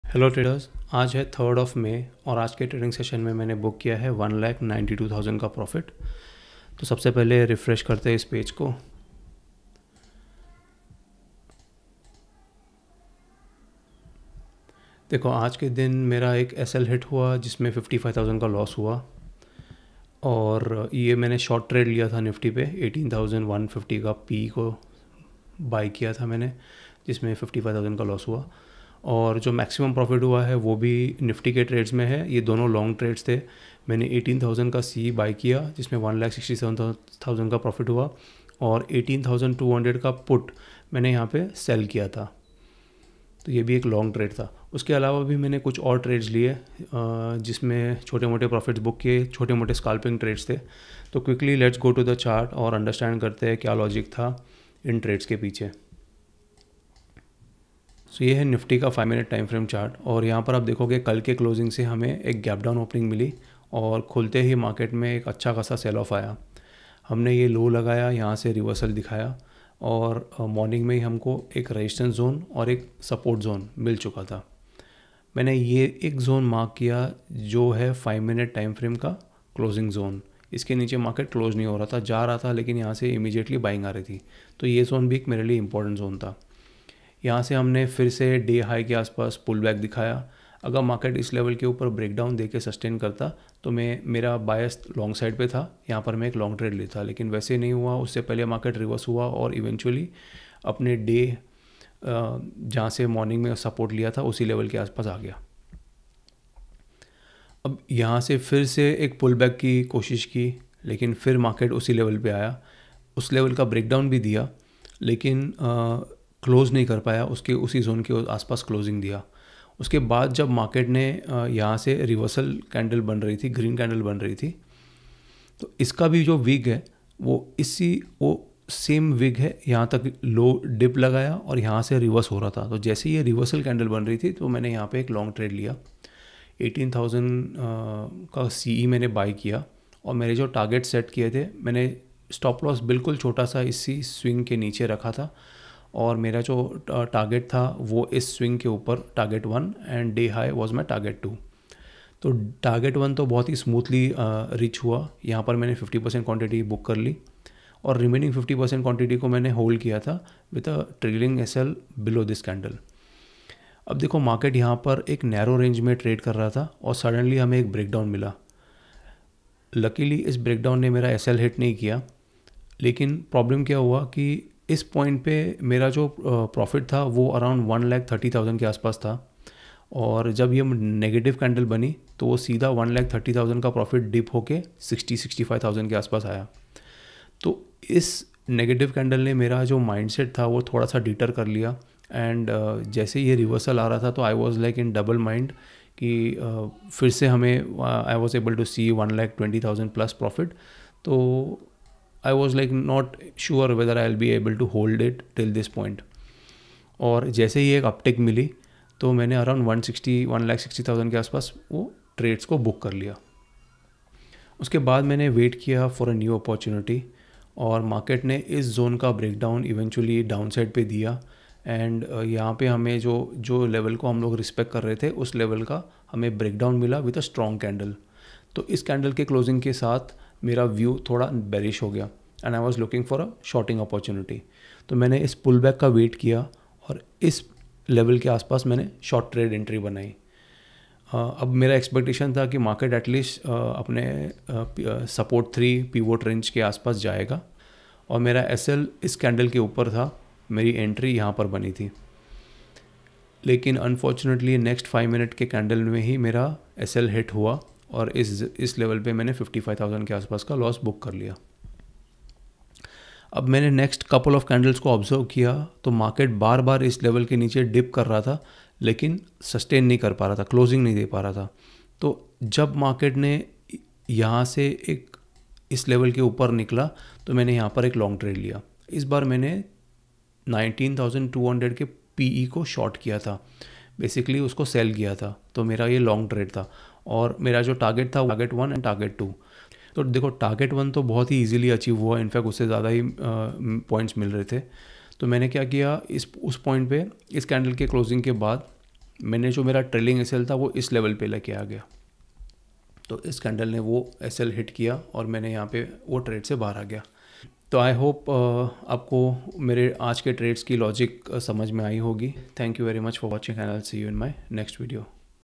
Option Trading | 03 May 2023 | Profit booked ₹1,92,407 | LifeStyle Trader
Option trading Intraday profits booked ₹1,92,407

To learn about different trading set-ups, price action-based trading, and swing trading.